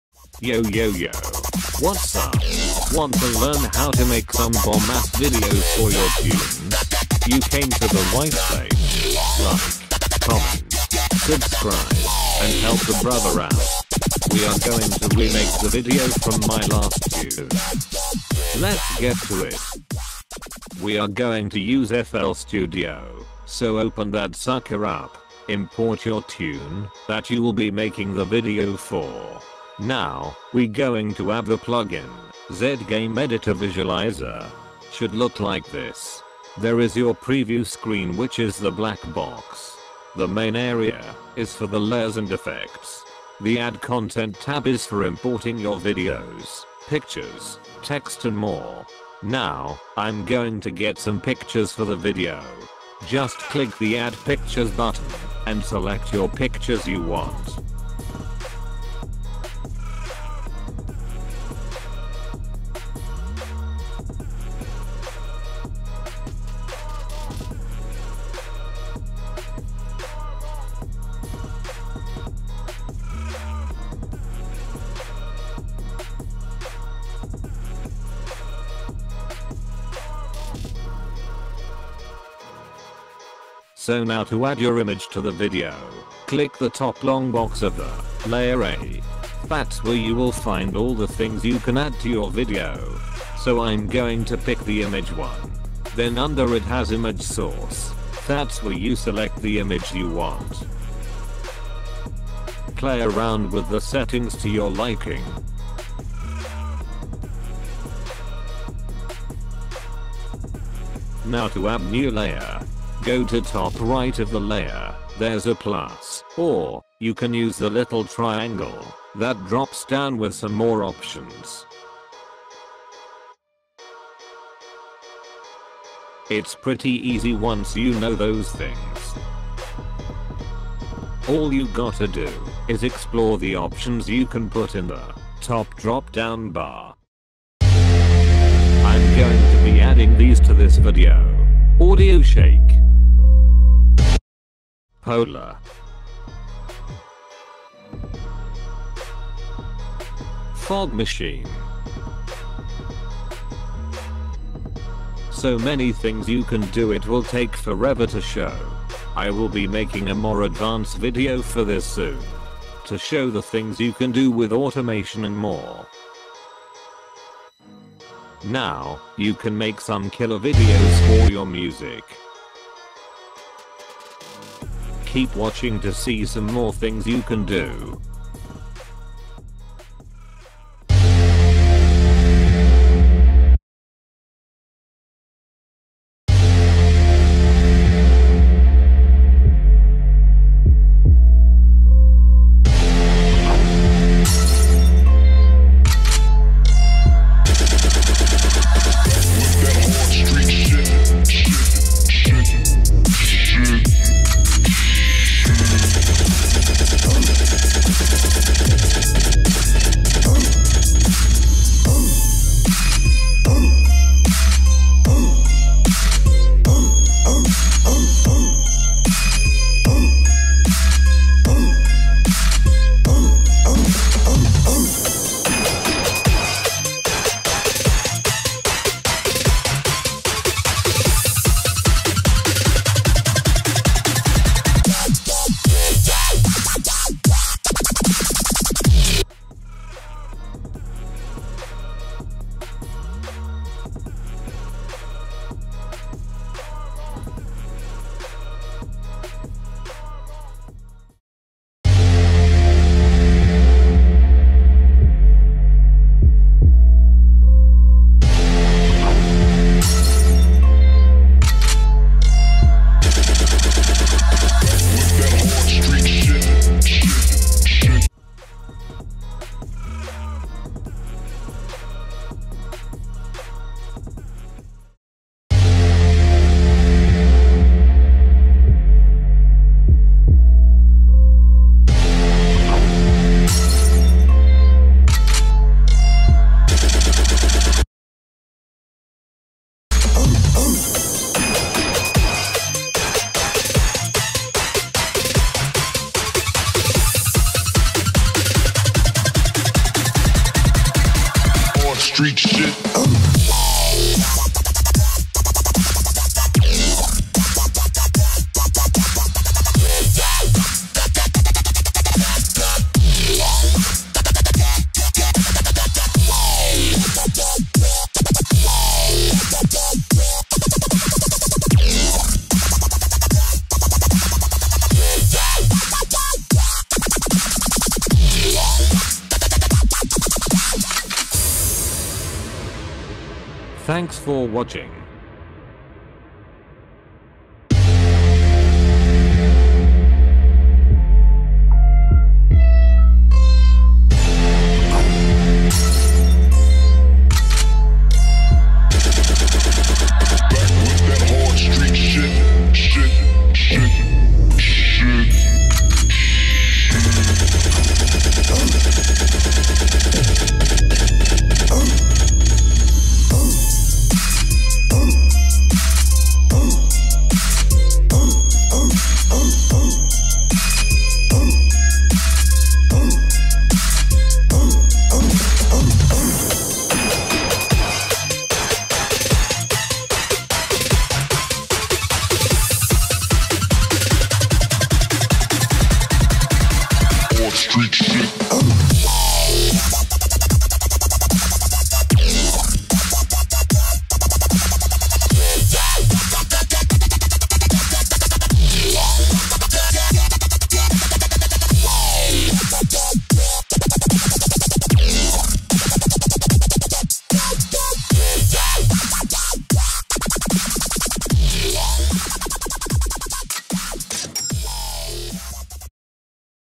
How To Make Videos Like DubstepGutter, Trap Nation, ETC! FAST & EASY! Audio Visualizer, Audio Shake!
Want To Make A Video Like All The Top Youtube Music Channels?
You KnoW The Kind Like DubStepGutter, Trap Nation, Rap Nation, Trap City, Drop The Bassline, ETC! I can go on forever! Well ill show you how to! Easy and Fast, you will have top notch videos to go with all your music after this video! ENJOY!

What You Need:
FL Studio12.5 & up!
ZGameEditor Visualizer (Included Plugin With FL Studio)
Your Fresh Banger Tune!
Some Pics For Background And Logo/Text!

The Main FX Used:
Peaks(Audio Visualizer)
 Audio Shake!

So Many Awesome FX To Make Some Kick Ass Videos!
Let Me Know If You Would Like More Tuts On This Subject!

GET IT!!!